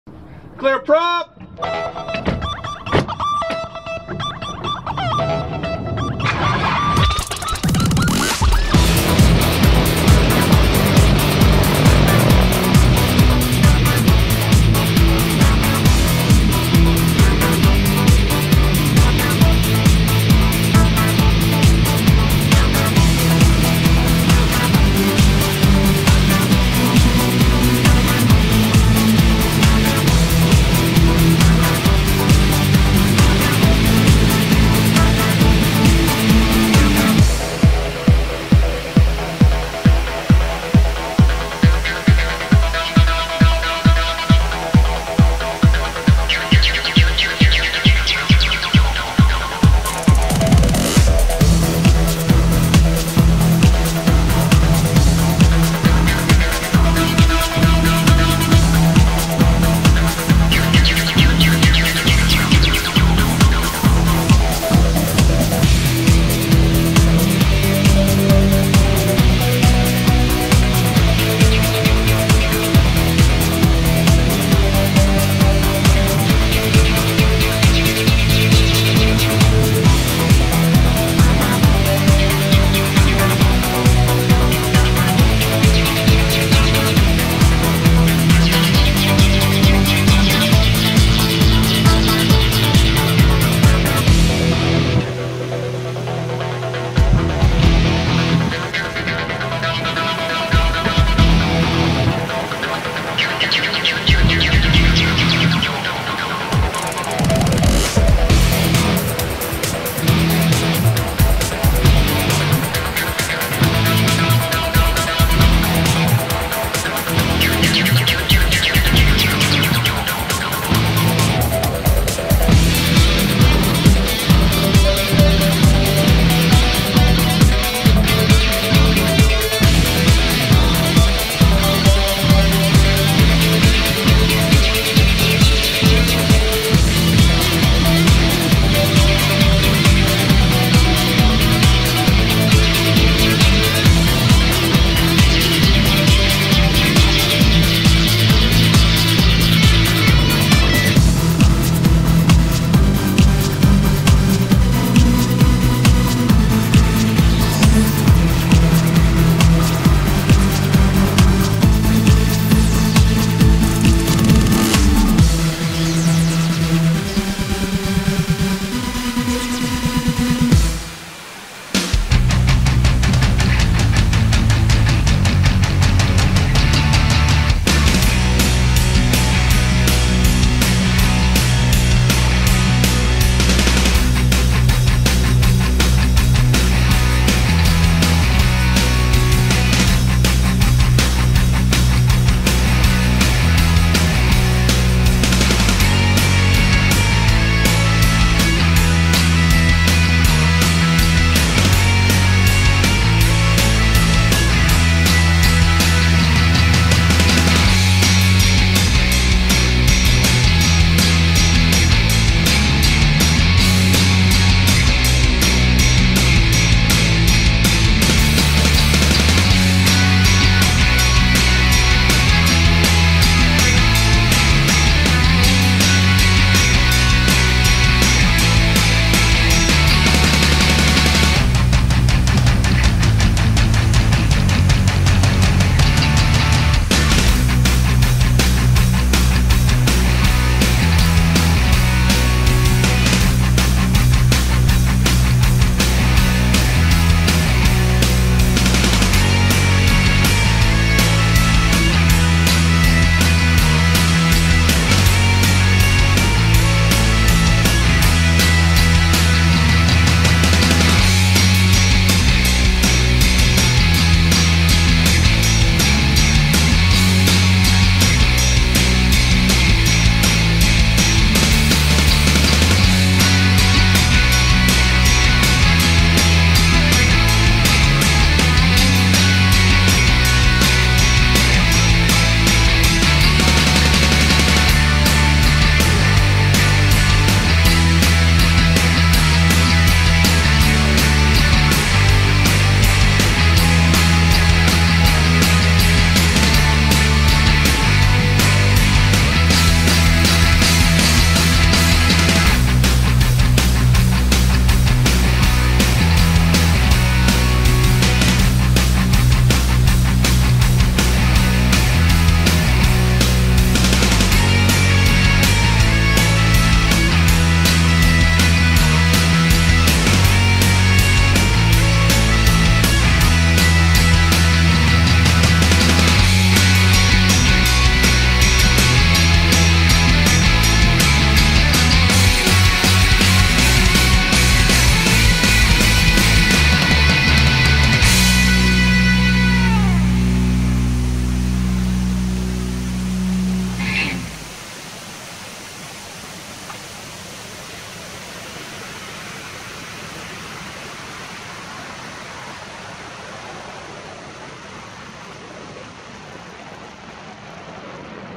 Nikon KeyMission 360 4k Video - Ride the Vomit Comet!
Set your YouTube viewer to 4k then look around as I perform intense aerobatics!  Try not to puke on your keyboard :-)

Shot on a Nikon Keymission 360 Camera